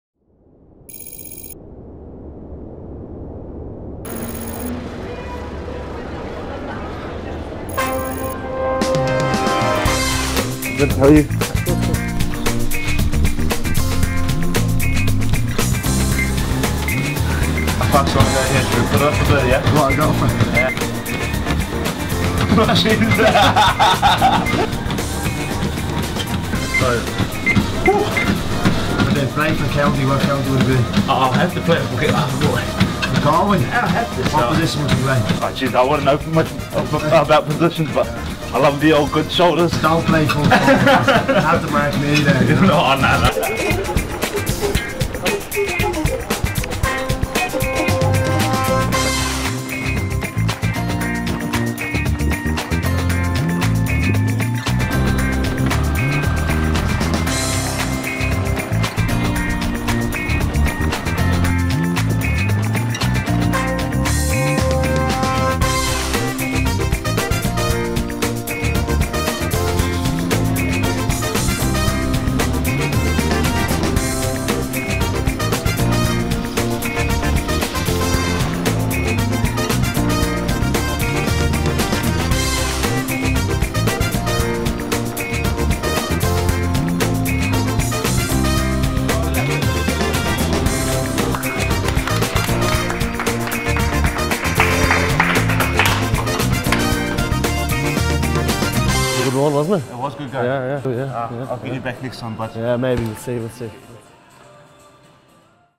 Bundee Aki vs Philly McMahon | Intersport Elverys; Get Ireland Running
Intersport Elverys challenged Bundee Aki & Philly McMahon to a race, to get Ireland running!

Check out who wins!